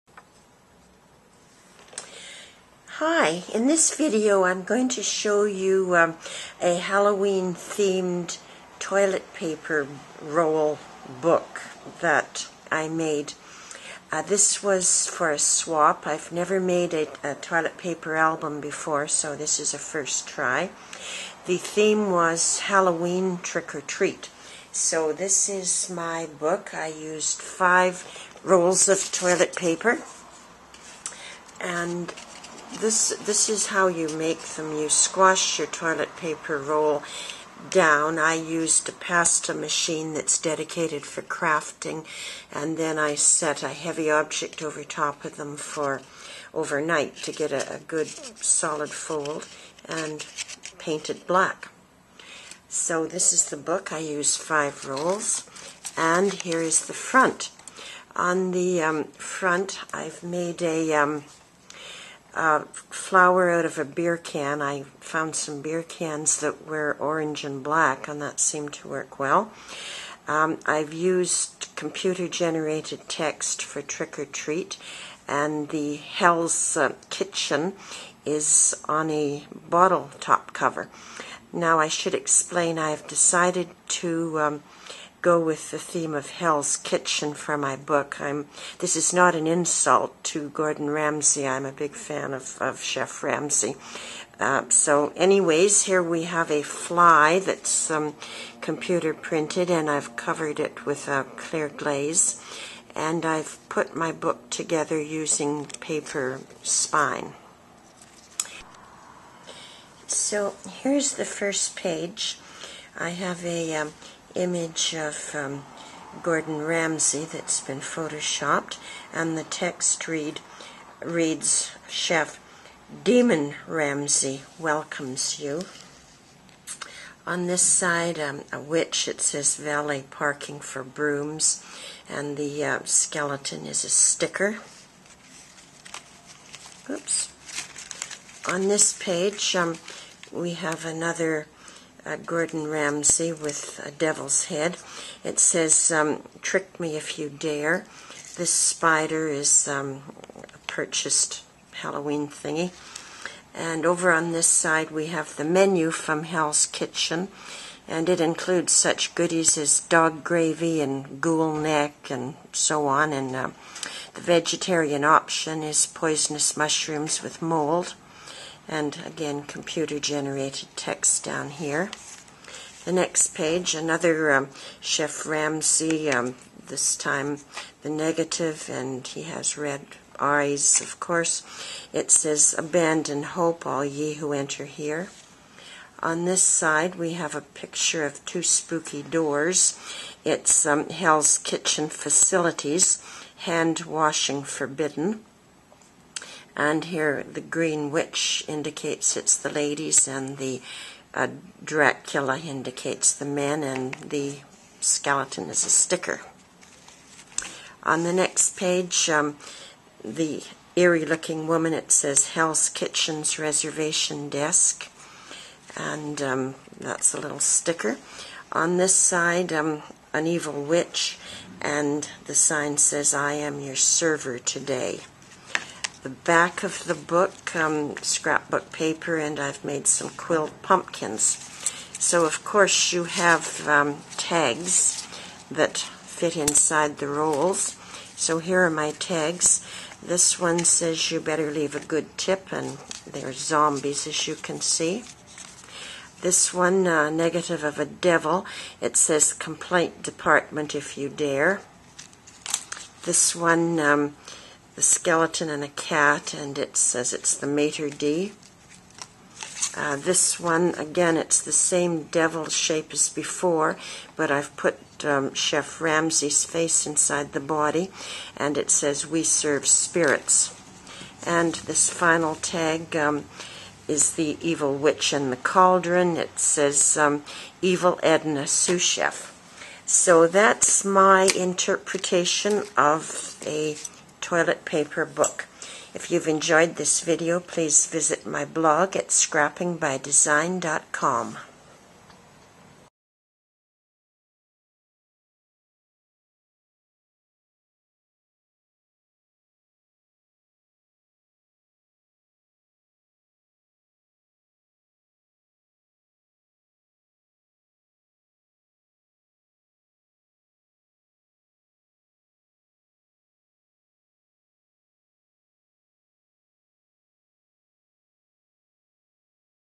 Halloween TP Mini Album
Trick or Treat at Hell's Kitchen with Chef Demon Ramsay. A mixed media
art project for Halloween theme.